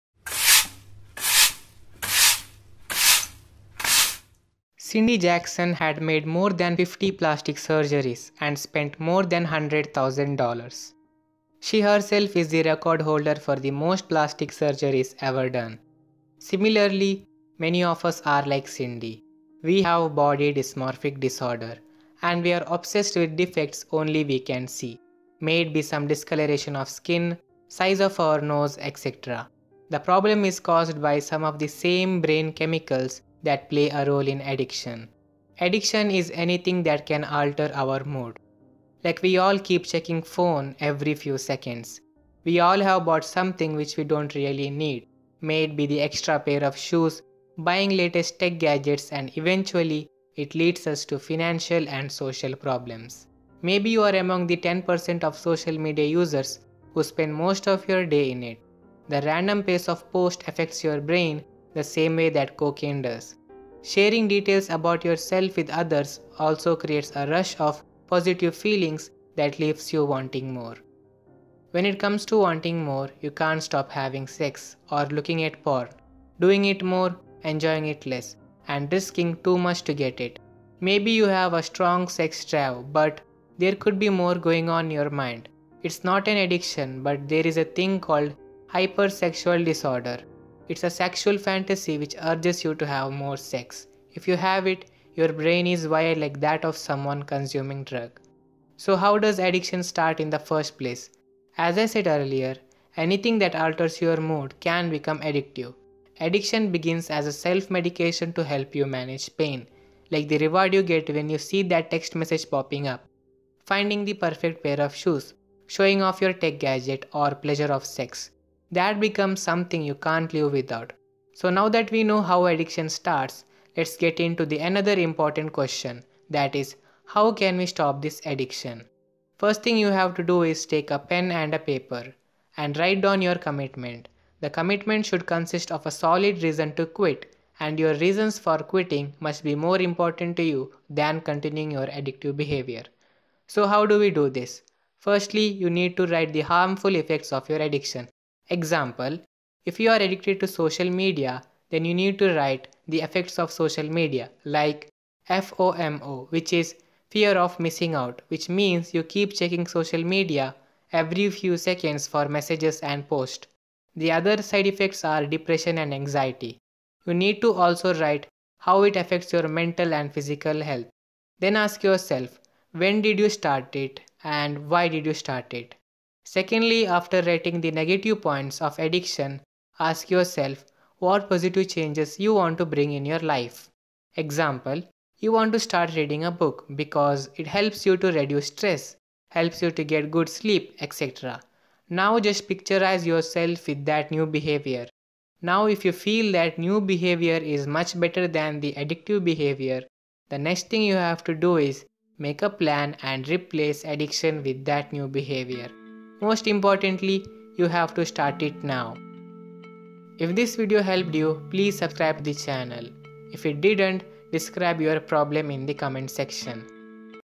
AID TO ADDICTION/ understand and CURE ADDICTION from its root| Mini Encyclopedia
HOW TO GET THE RID OF ANY ADDICTION

  Firstly we cannot cure Addiction as its not a disease. It’s a biopsychosocial disorder. That’s the reason why people who are addicted to drug need a professional help. Drugs like cocaine don’t need a dopamine to get pleasure instead it forms its own chemical structure which is similar to dopamine. That’s the same case with cigarette.
But there is a possible solution for other addictions. For those, the only thing you can do is replace it with a new kind of addiction. Something which is much better than you have now.
If your addicted to a movie start watching documentaries. If your addicted to social media hang out with friends. But this is only possible if you are aware that your in addiction .
As there is a saying “To solve a problem you need to identify the problem”. Addiction is not something which can be solved in  a day. It takes time a lot of time. Keep hope and do what is best.
 I hope this video was helpful. Like this video if you want to get rid of addiction as early as possible. Describe your problem in the comment section we might give you a  possible solution.